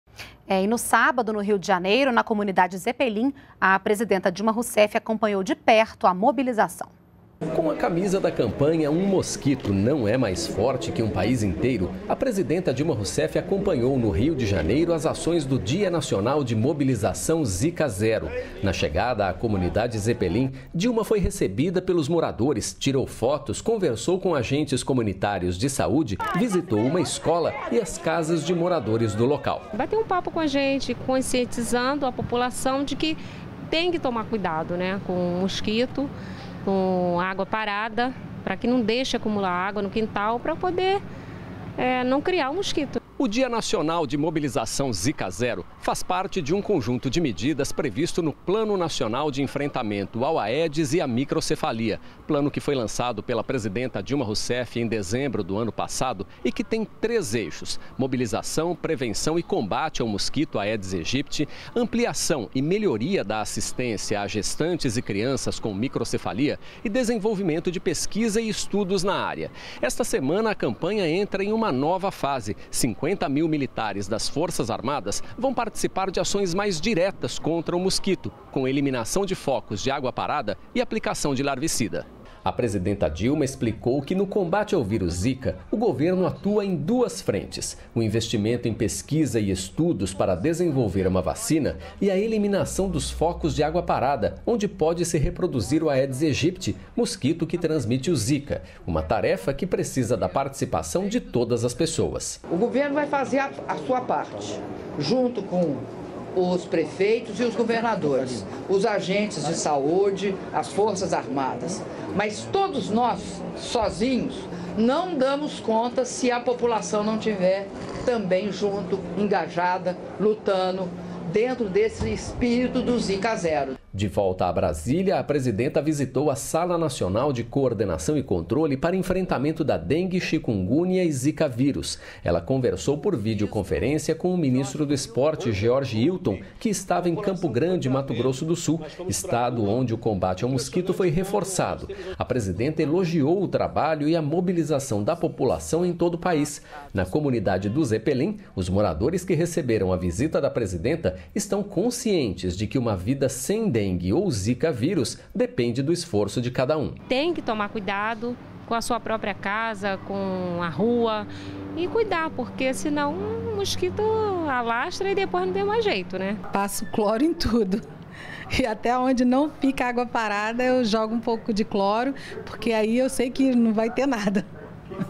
Presidenta Dilma acompanha ações do Dia de Mobilização Zika Zero no Rio de Janeiro
NBR NOTÍCIAS - 15.02.16: A presidenta Dilma Rousseff acompanhou no Rio de Janeiro as ações do Dia Nacional de Mobilização Zika Zero, realizado no último sábado. Dilma visitou a comunidade Zeppelin e conversou com a população sobre os cuidados necessários para evitar a proliferação do mosquito Aedes aegypti.